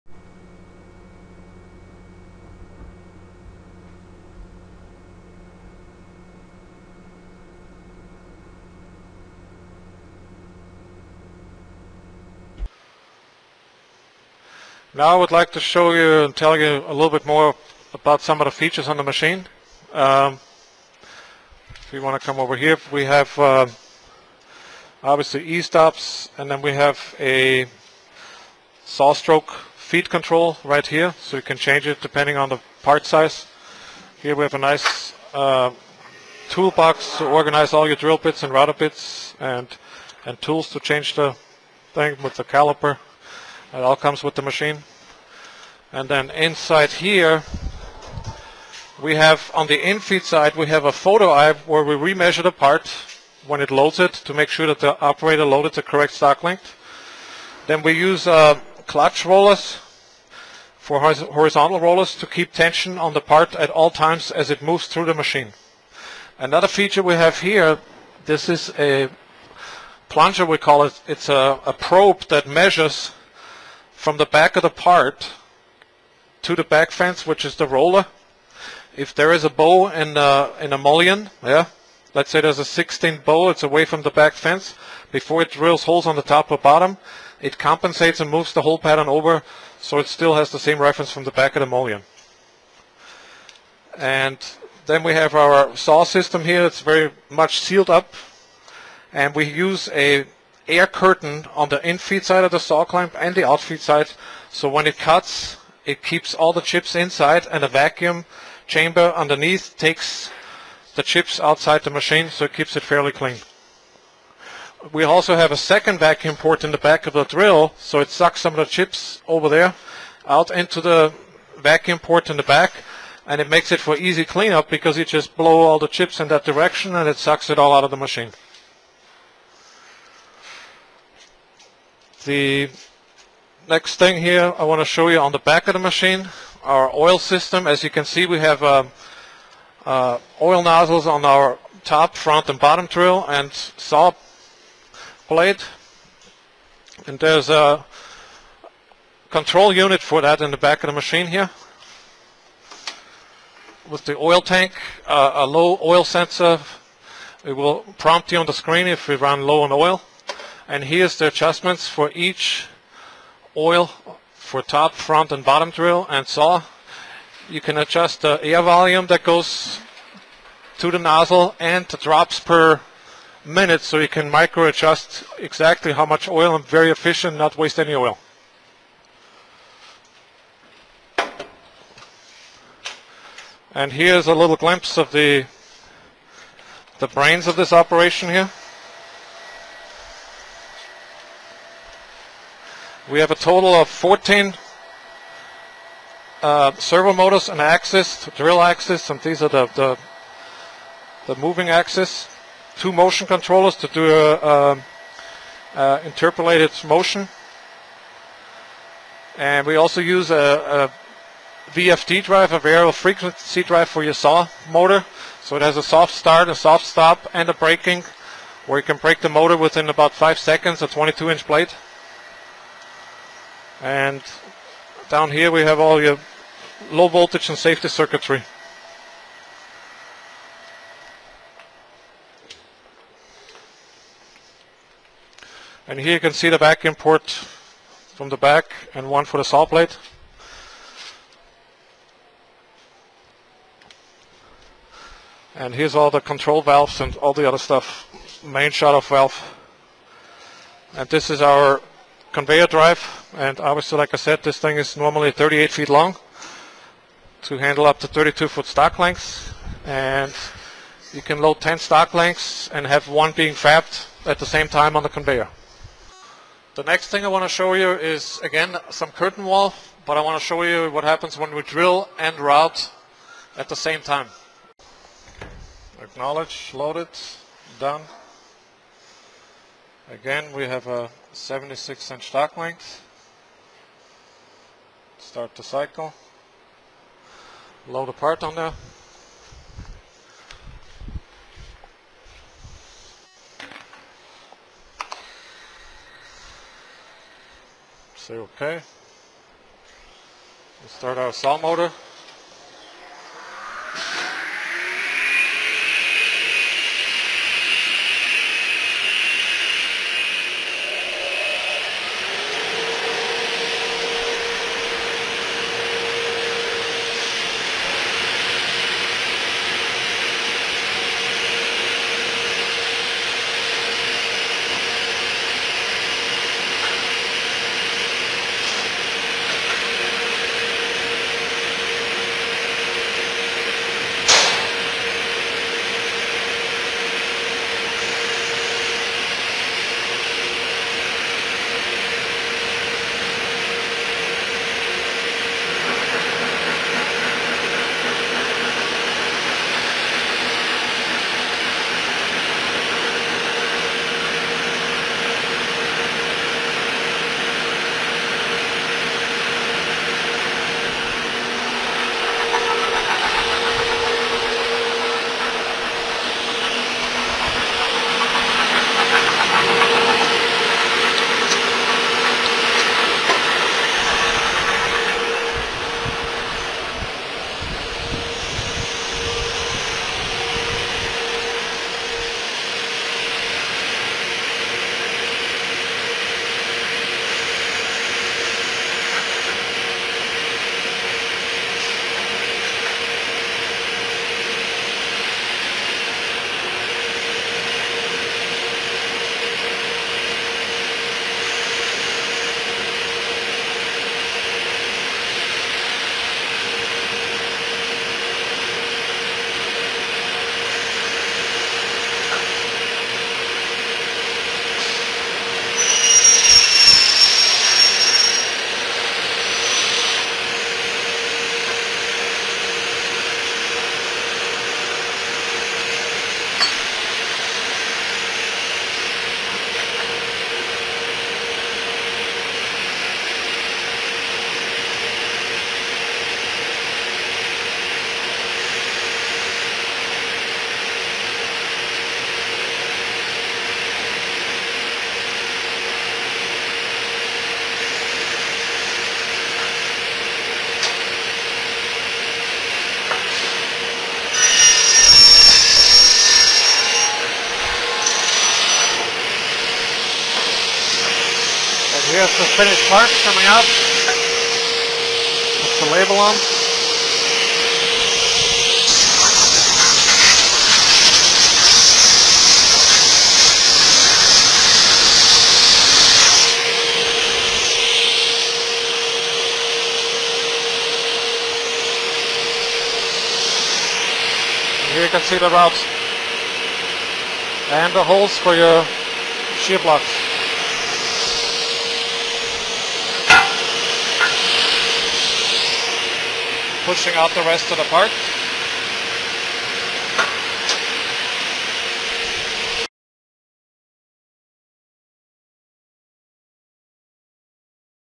RhinoFAB Machinery - Drilling and Routing
RhinoFAB Machinery is an innovative approach to producing storefront and curtainwall elevations..